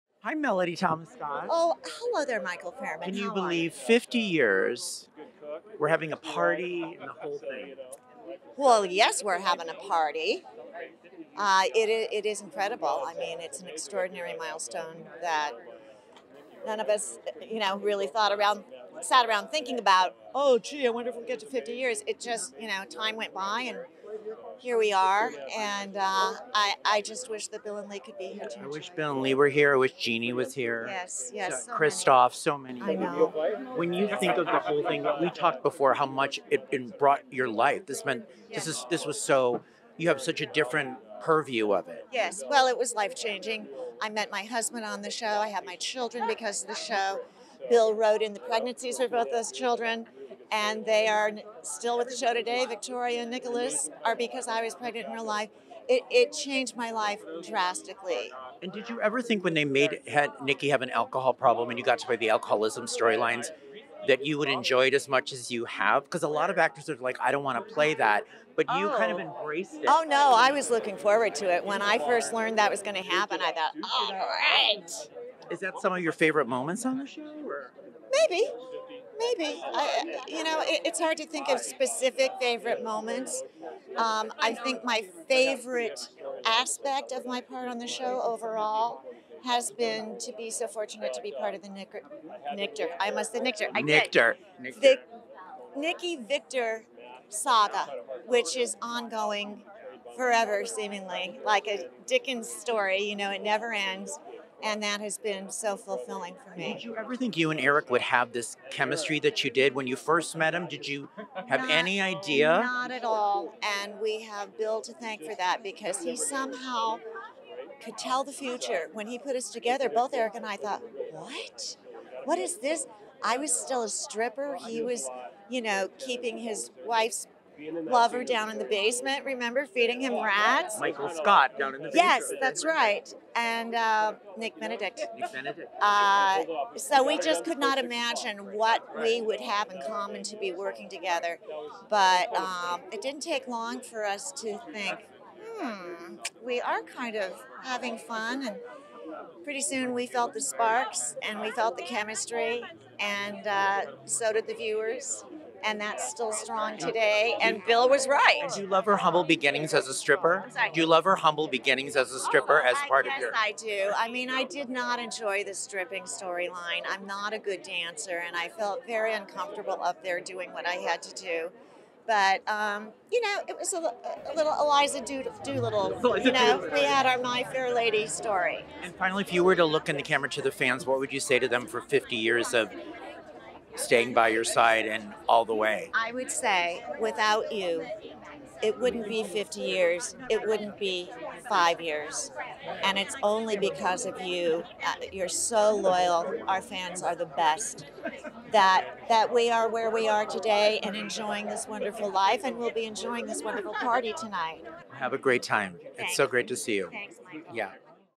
Melody Thomas Scott Interview - Y&R 50th Anniversary Party
The Young and the Restless Melody Thomas Scott (Nikki) chats with Michael Fairman at the red carpet at Y&R's 50th anniversary party.

Melody discusses how working at Y&R changed her life both personally and professionally, being part of the most famous and enduring couple in the history of the show, Nikki and Victor, and getting to play a drunk Nikki, and more.

Share your thoughts on our moments with Melody in the comment section below, and make sure to 'subscribe' to the Michael Fairman Channel for more Y&R 50th anniversary red carpet interviews and upcoming celebrity features.